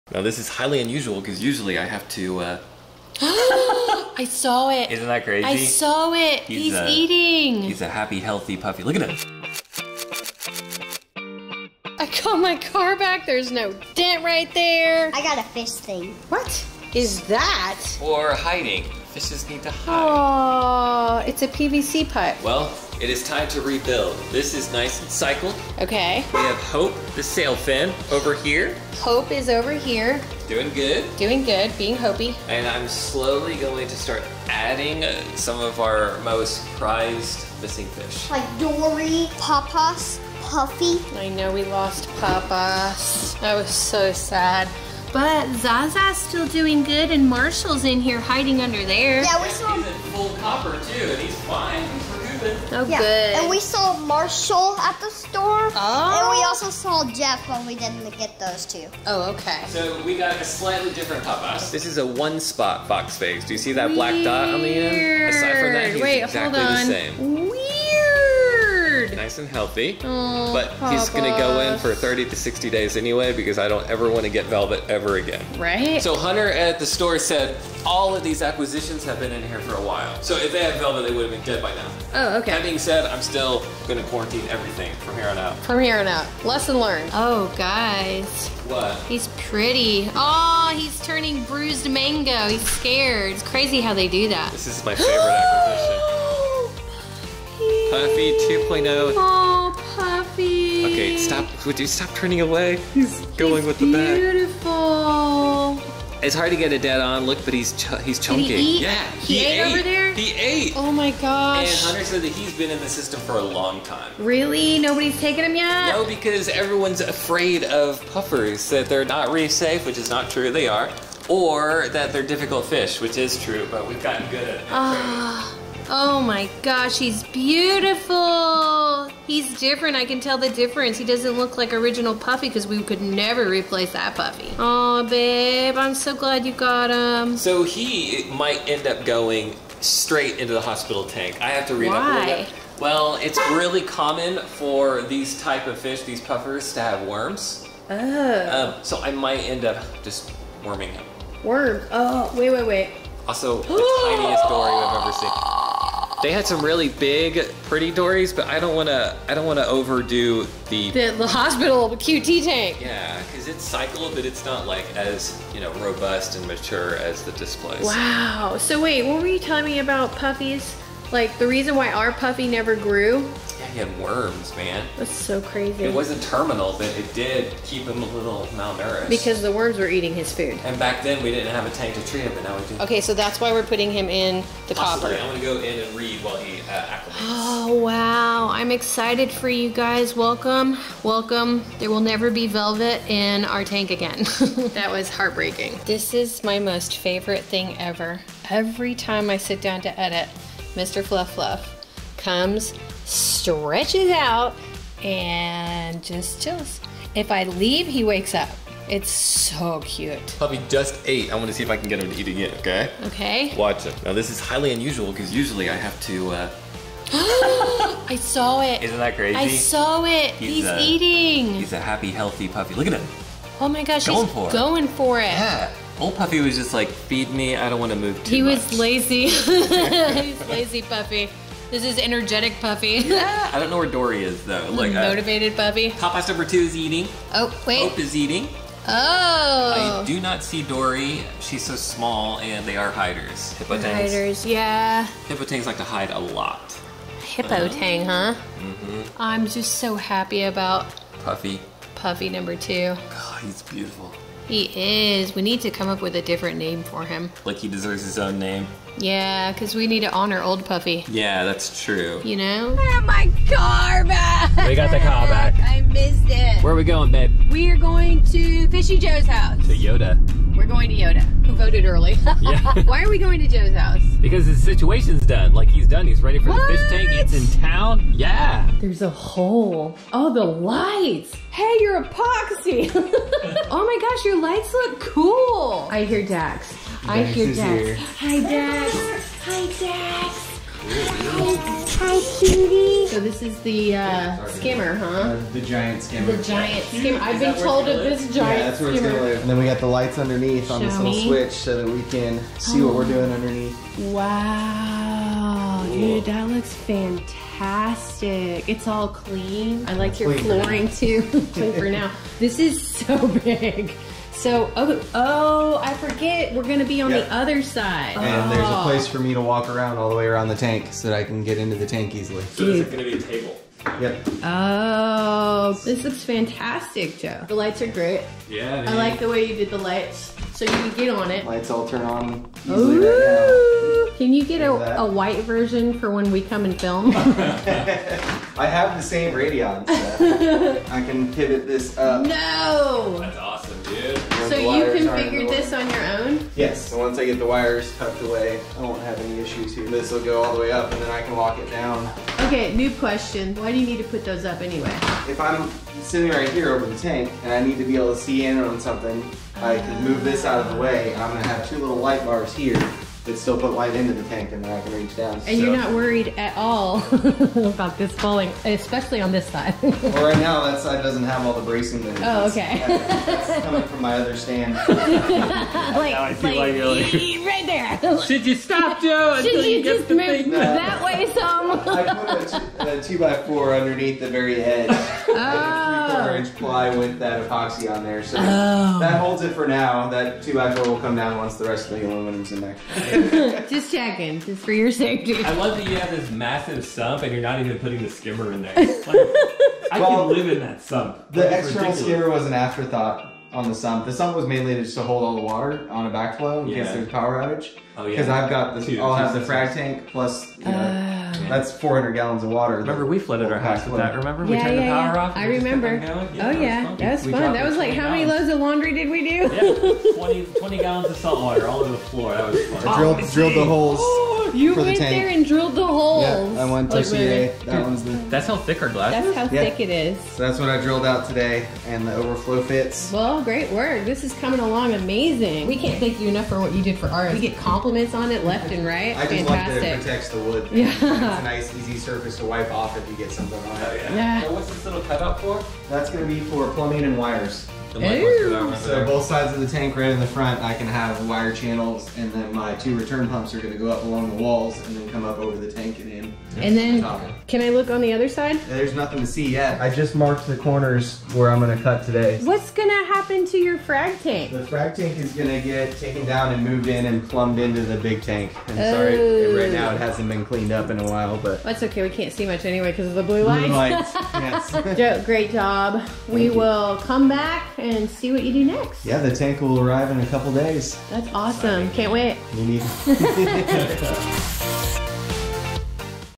NEW PUFFERFISH!! We Need A Name! HELP US NAME HIM
Today on The Izzys we introduce a new Spiny Box Puffer to quarantine! R.I.P., Puffy! Help us name his replacement!